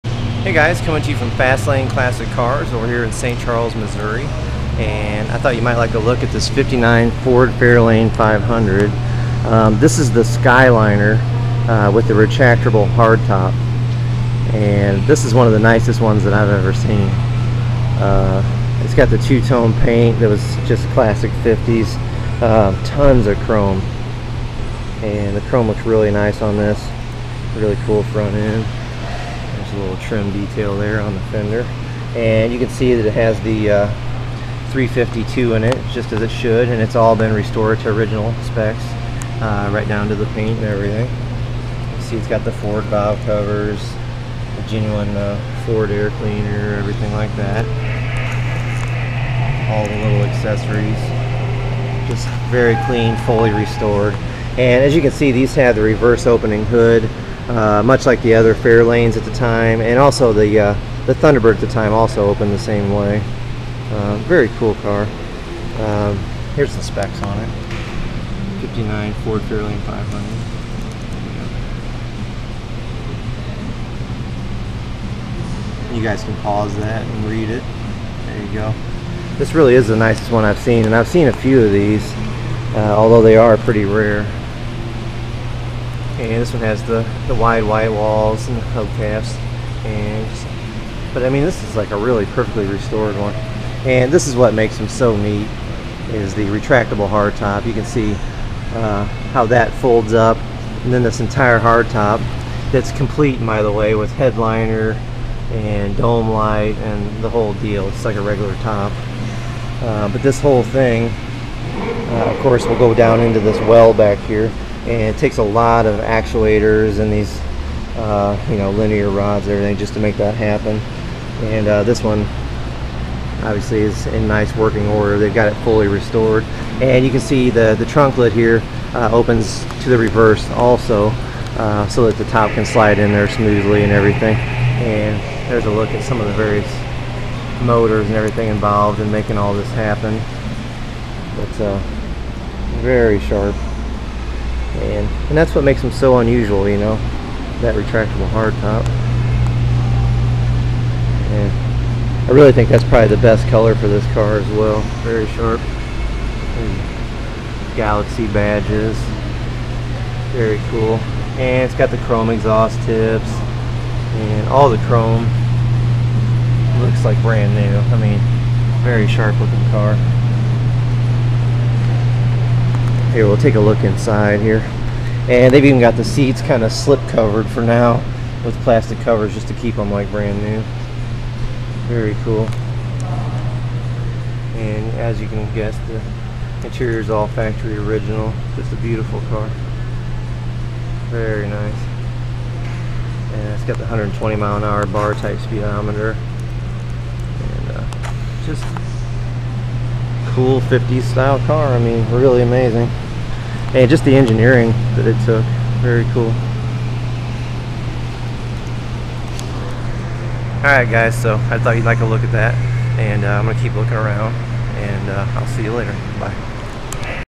1959 Ford Fairlane 500 Galaxie Skyliner Retractable Hardtop - Fully Restored Classic Car
352c.i. 4BBL V8 (July 25, 1969 Date Coded Block), Automatic (Cruise-O-Matic) Transmission (Trans Code-4), 9in Rear w/ 3.10 Gear Ratio (Axle Code-1), Power Steering, Power Brakes, Factory AM Radio (Town & Country), Floor Mats, Dual Exhaust, Show Quality Chrome, Seats Still in Plastic Seat Covers, Appears to be Original Interior w/ Newer Carpet, Power Retractable Hard Top, Turquoise & Colonial White Exterior & Interior, Factory Steel Wheels & Hubcaps w/ Wide White Tires, Owners Manual, Very Nice Undercarriage, Gorgeously Restored & Preserved! One of the Finest Retractable Hardtops Around!!